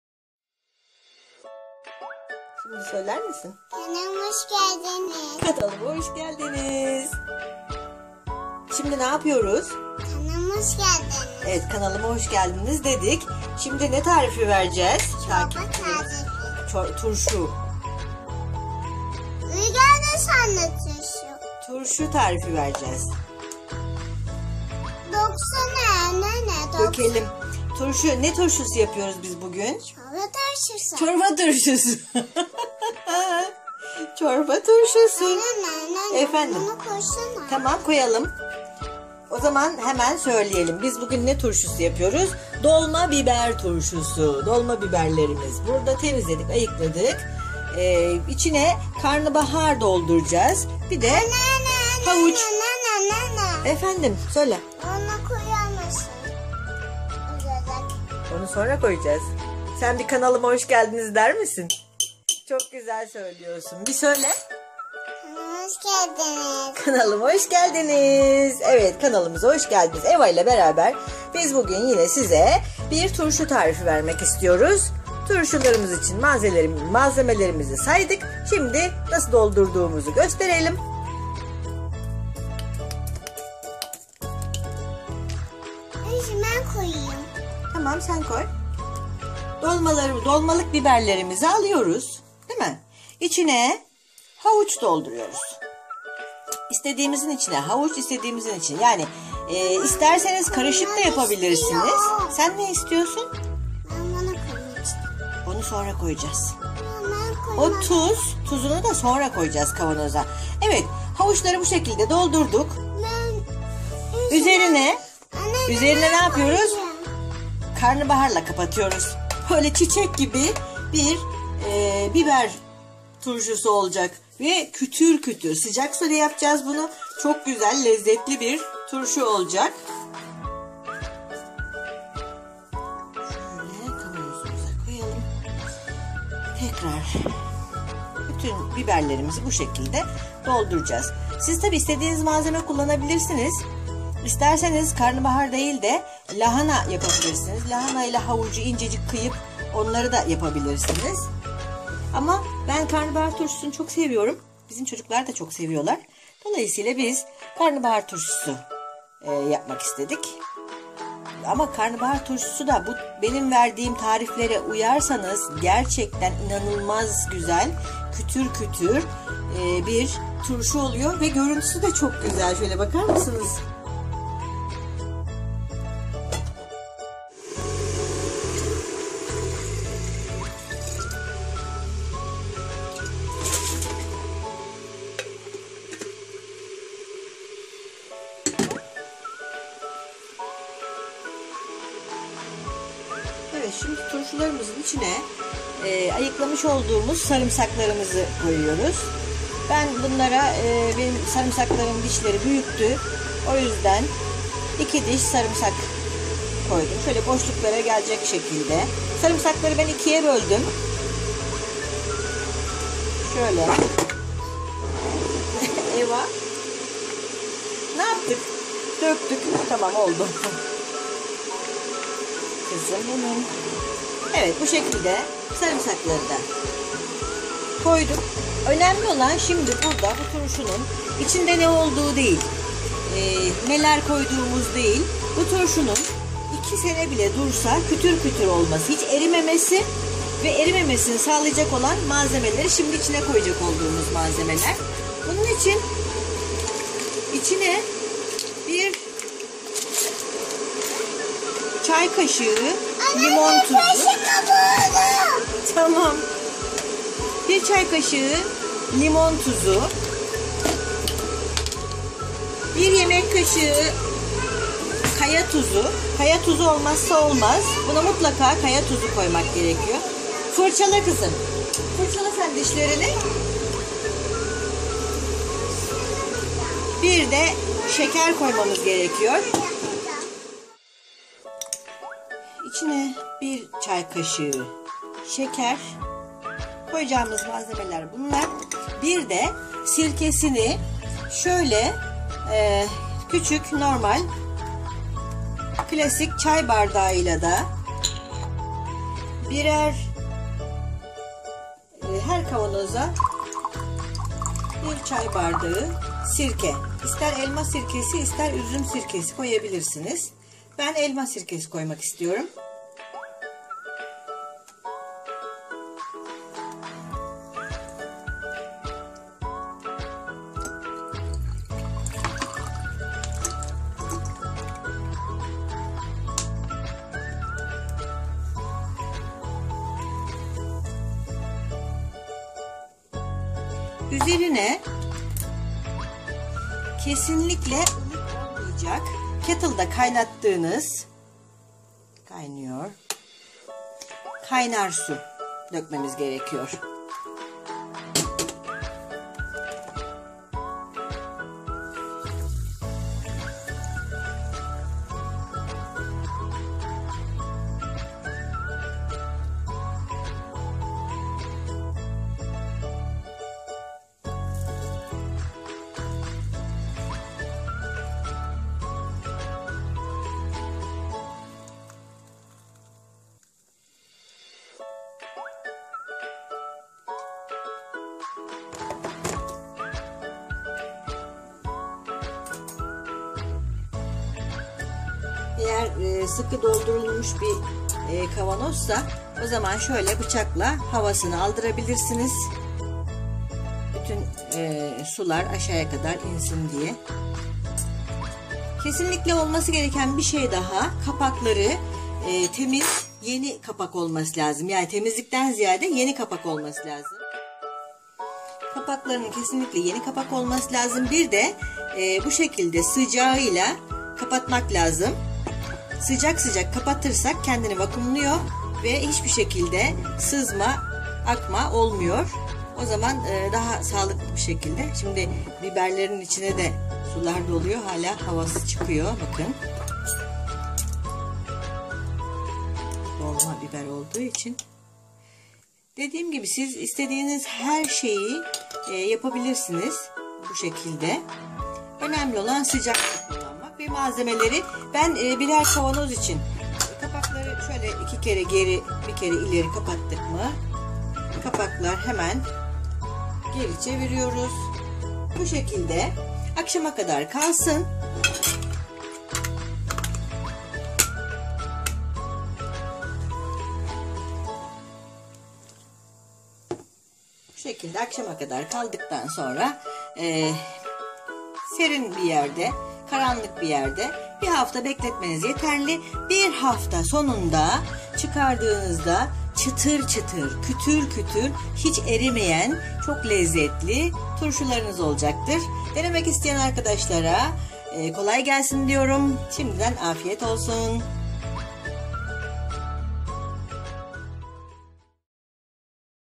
ÇITIR ÇITIR BİBER TURŞUSU NASIL YAPILIR/BIBEF TURŞUSU TARİFİ
DOLDURULMUŞ BİBER TURŞUSU NASIL YAPILIR, ÇITIR ÇITIR BİBER TURŞUSU TARİFİ İÇİN GEREKLİ MALZEMELER
1 küçük karnabahar
10 tane dolmalık biber
3 tane havuç
2 tane 1 lt.lik kavanoz

1 kavanoz için 5 doldurulmuş biber
1 yemek kaşığı kaya tuzu
1 cay kasigi limon tuzu
1 cay kasigi şeker
1 cay bardagi sirke
Üzerine kaynamis sıcak su
Tüm malzemeler  kavanoza konur sıcak su ilave edilir kavanoz daki hava çıkarılır. Sıcak sıcak kapaklar kapanır vakumlanma sağlanır ters çevrilip 5 gün bekletilir .Bu ölçüler le istediğiniz her sebzeden çıtır çıtır  turşu yapabilirsiniz.💜👌